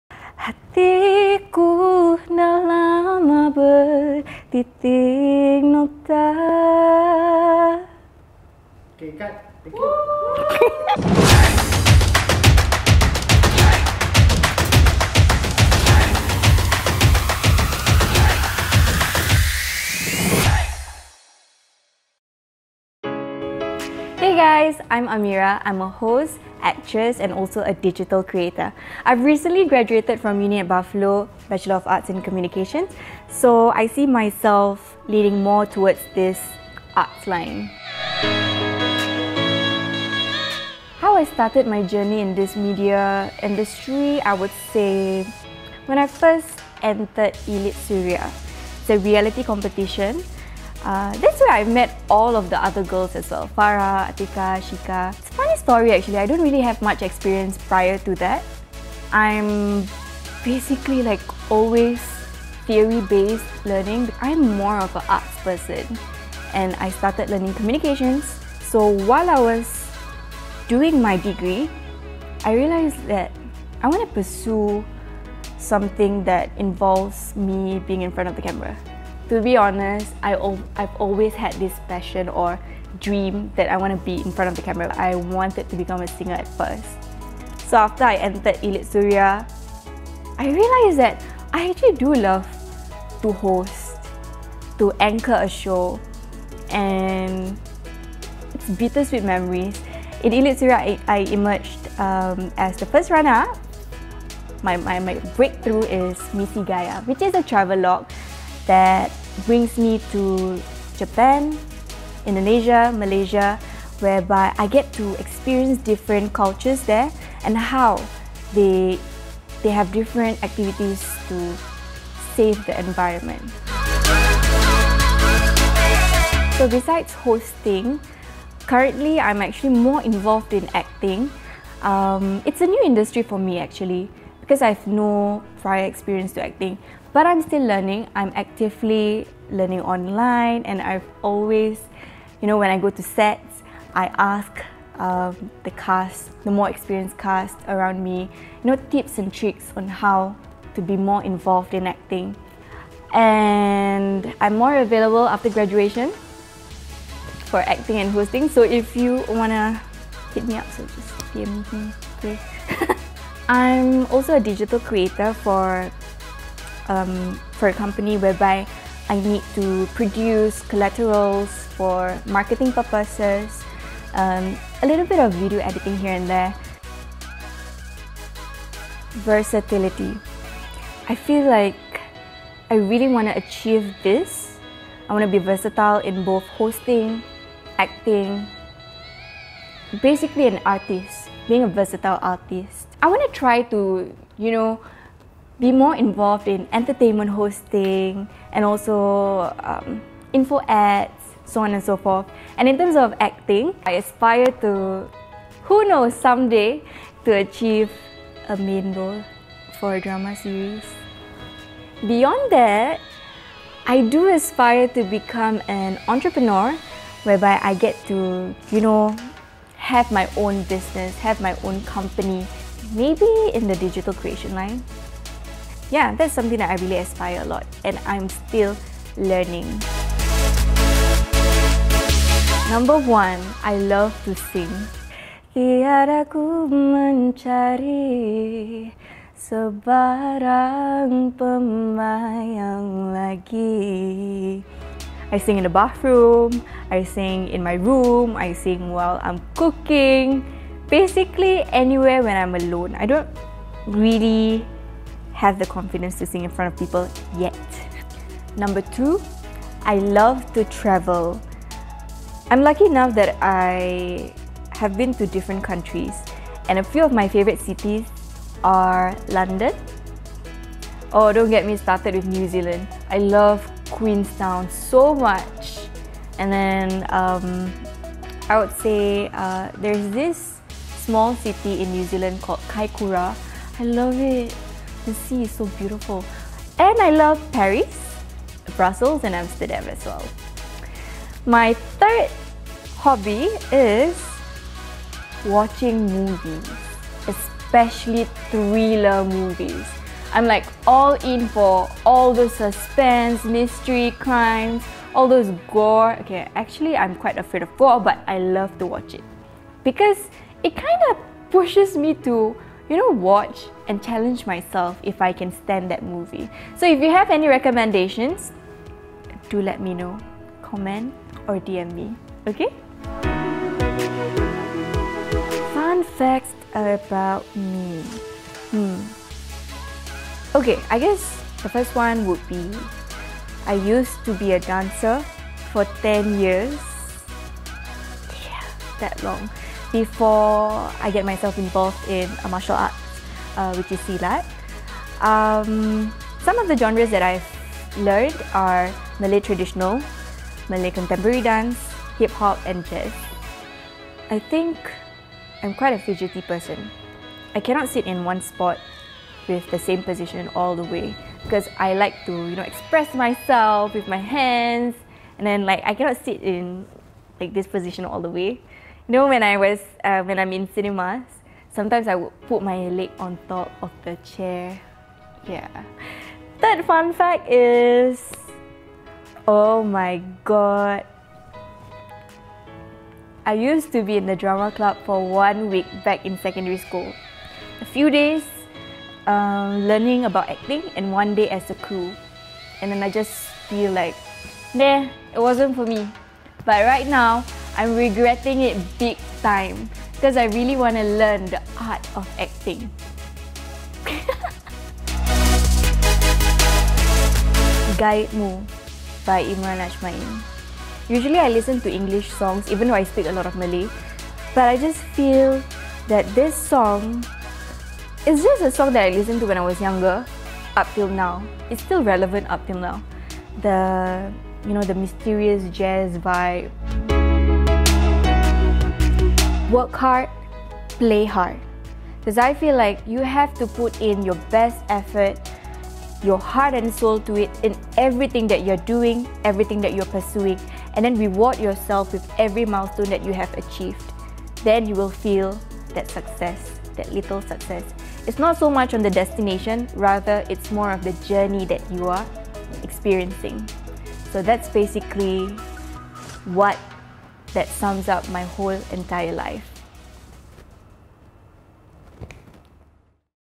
Get To Know: Amyrah Mustafa
Introducing 1/4 of Vibe Check: Amyrah Mustafa

Watch this space as 4 girls leave their comfort zones and take on quirky challenges! 🙌🏼
Have a challenge to dare us? Write down in the comments section down below! We want to hear from you! 🔥🔥🔥